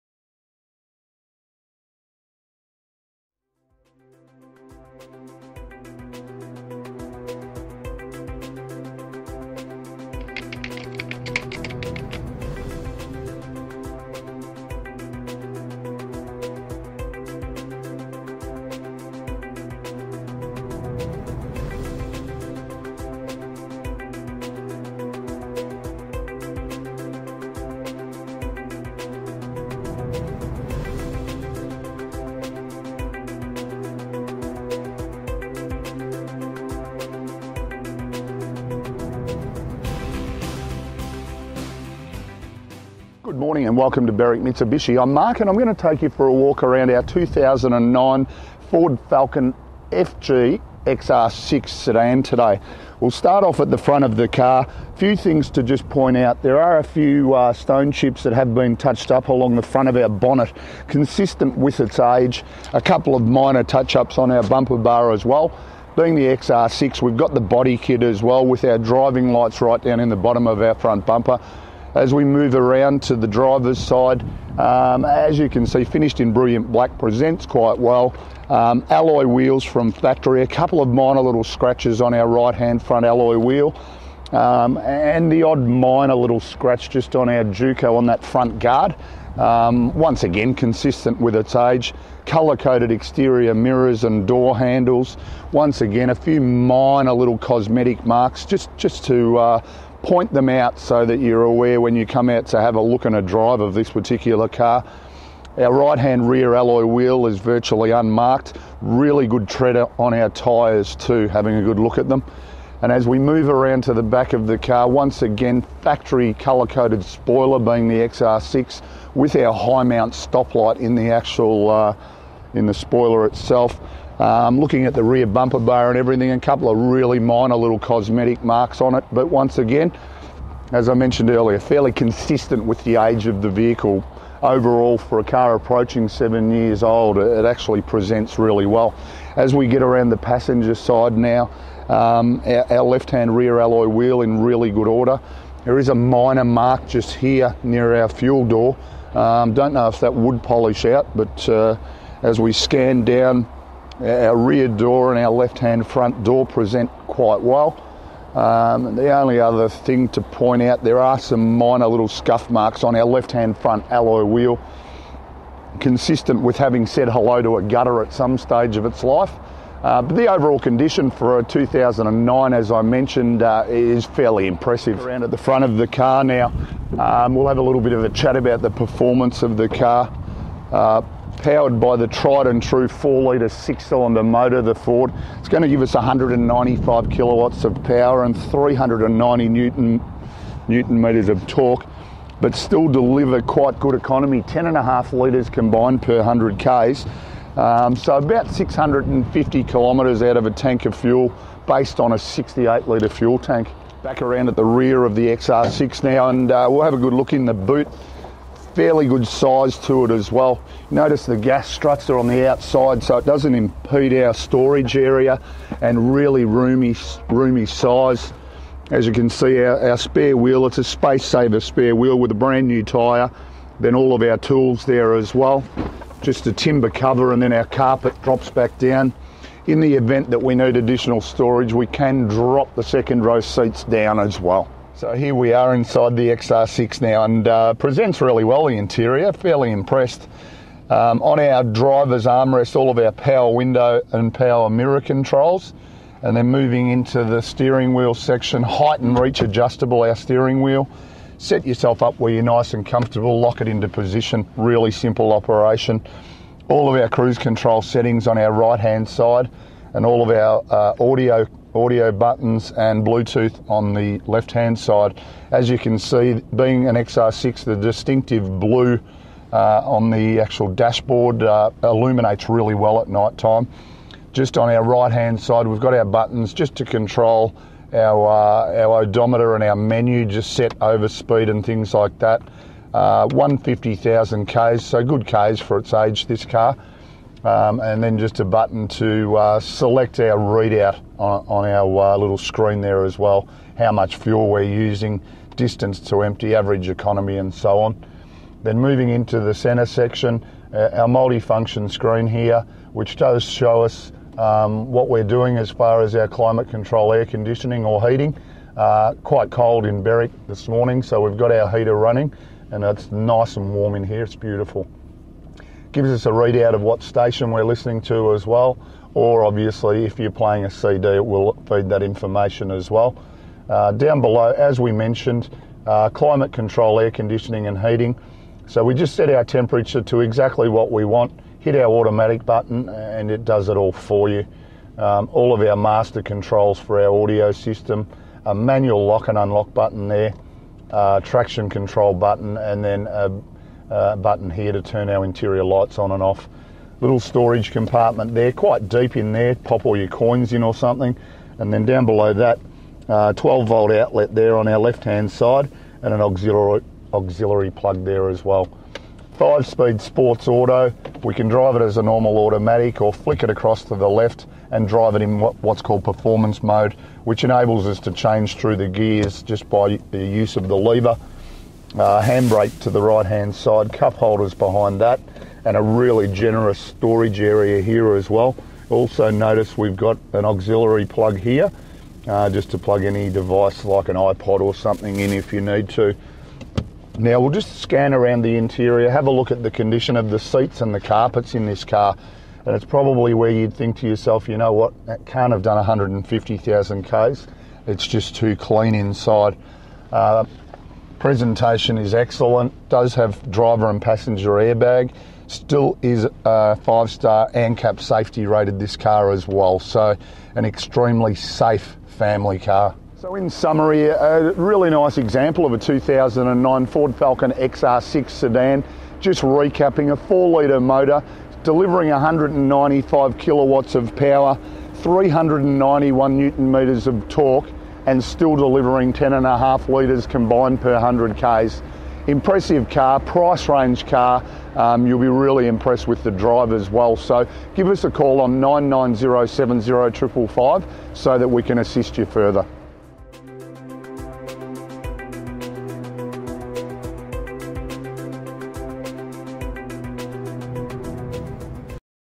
B5296 - 2009 Ford Falcon XR6 FG Auto Review
A video walkaround tour of the B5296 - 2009 Ford Falcon XR6 FG Auto Review Presented by Mark Mcgregor Used Car Sales at Berwick Mitsubishi,